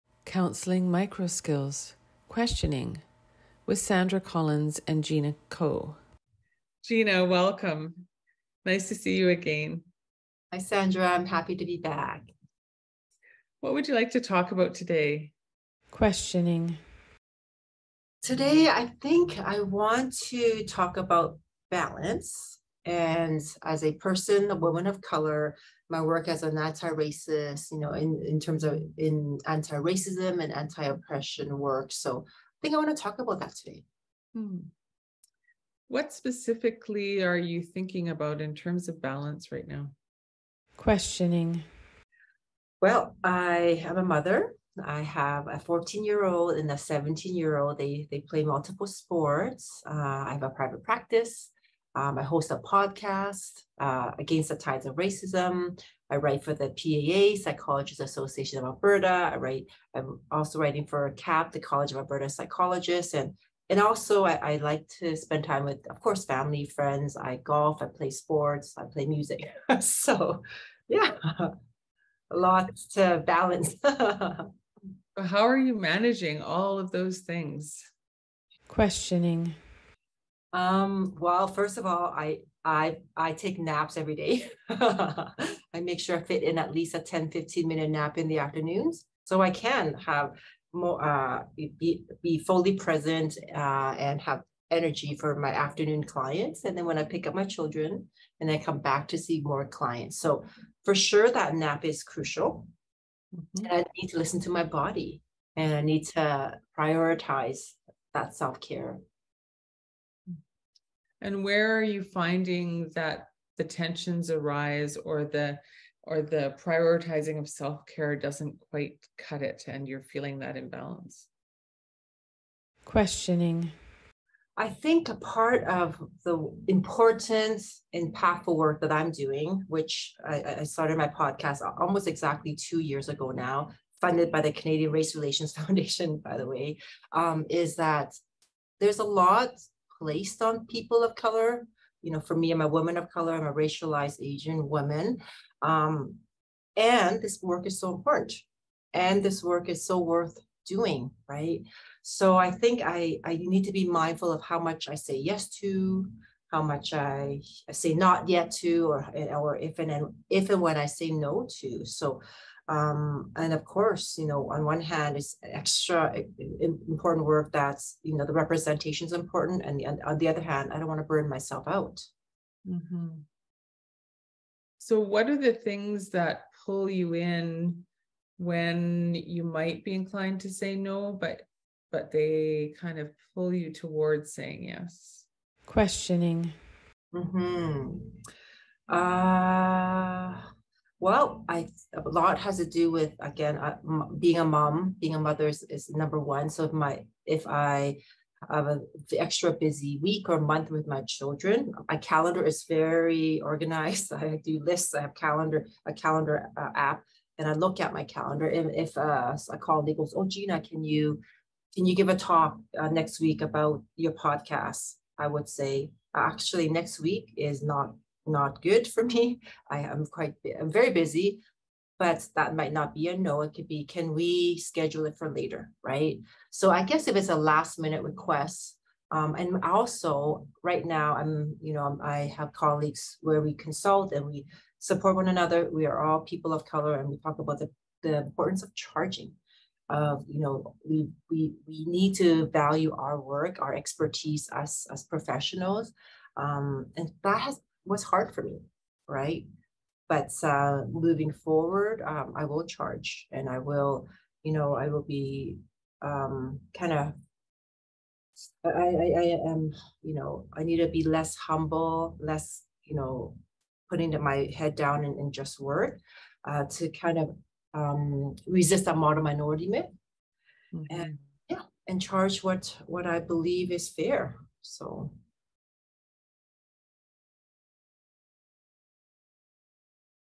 Counselling Microskills: Questioning
Counsellors use questioning as a way of inviting clients to share their feelings, perspectives, beliefs, and other aspects of their experiences. Quesetioning typically begins with an interrogative (i.e., what, where, when, how, why). These types of questions tend to encourage clients to expand on their ideas and are consider more open than questions that lead to yes/no responses. We debrief this skills demonstration in this video